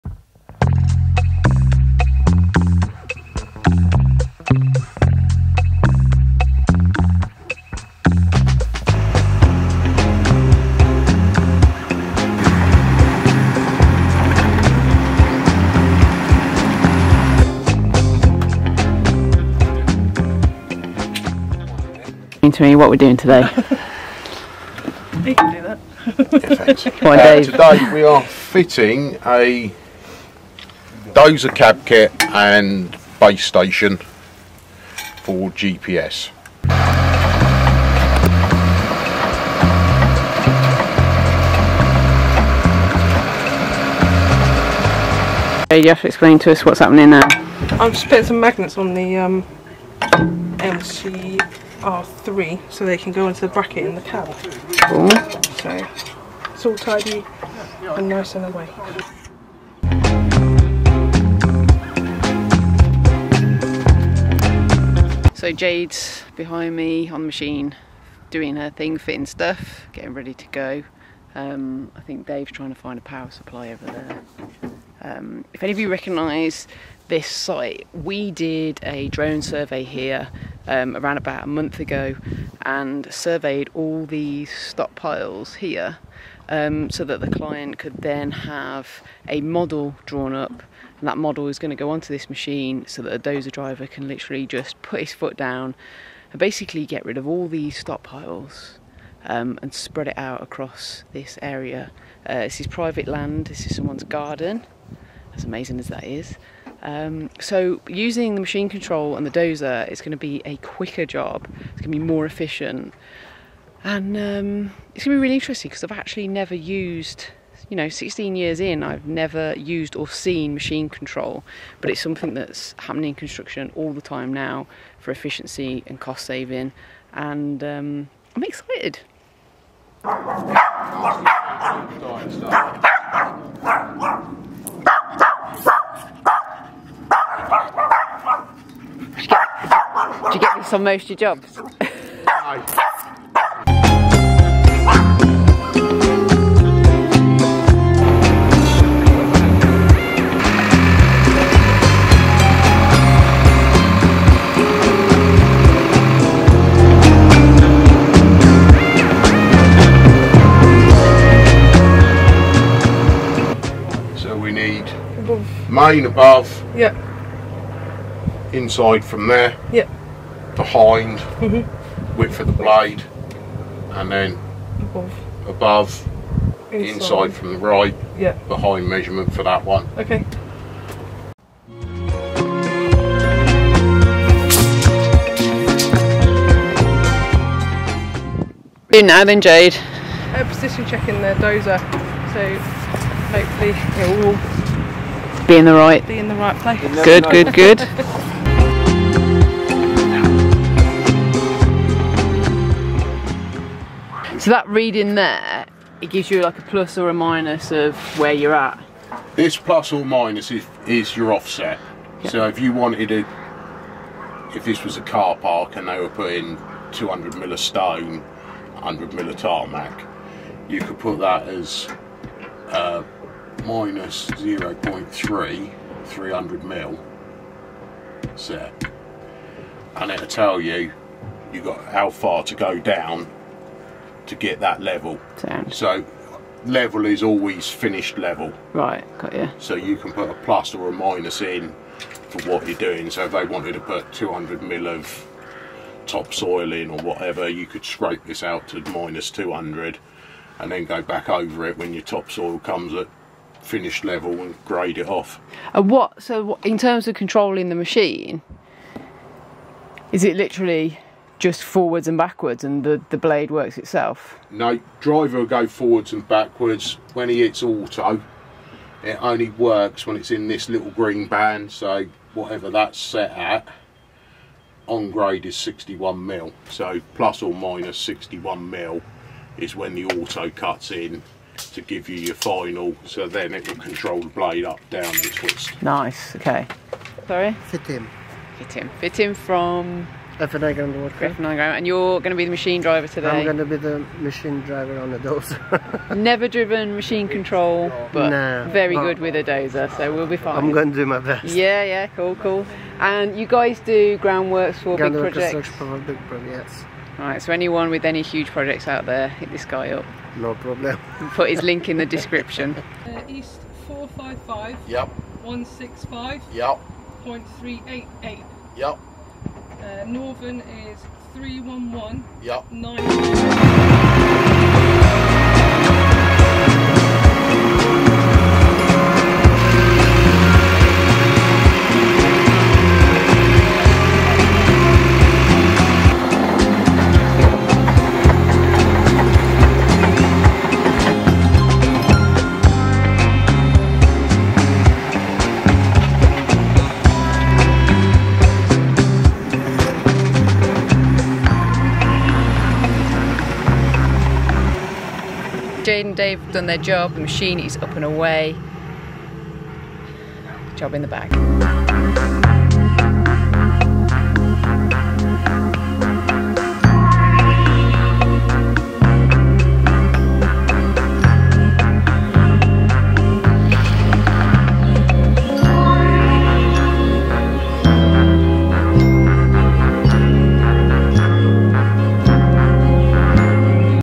My Introduction to Machine control on a Dozer!
My Introduction to Machine control on a Dozer, I see firsthand how a dozer is set up with Topcon Machine control. Machine control is getting really common in construction and earthworks, so I wanted to see how easy the set up is and how it works.

Special thanks to Progress hire for the dozer